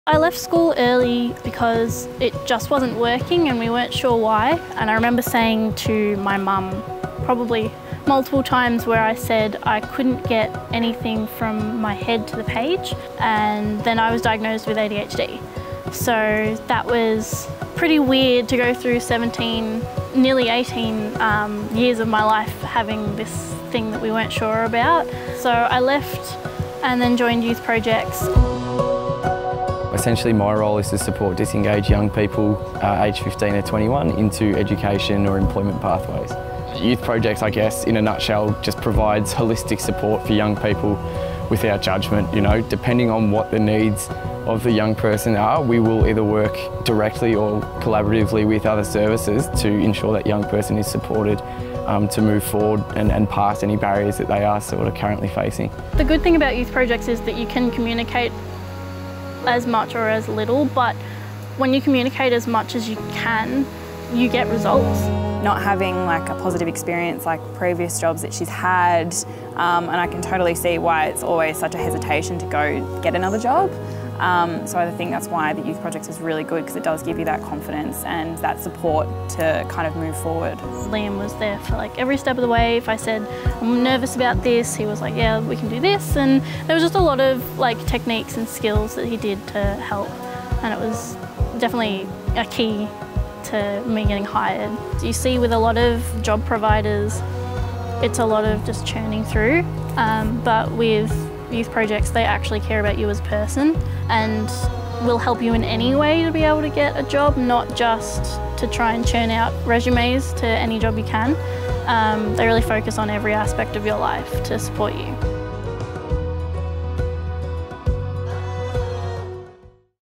Erin's Story
Our team of employment and training specialists work alongside hundreds of young people to create tailored plans to not only see young people employed, but equipped with the skills and confidence to succeed.

We know that social and economic participation is key to a young person's success, but we also know that young people face multiple barriers to employment.

This is one of those young people. This is Erin's Story.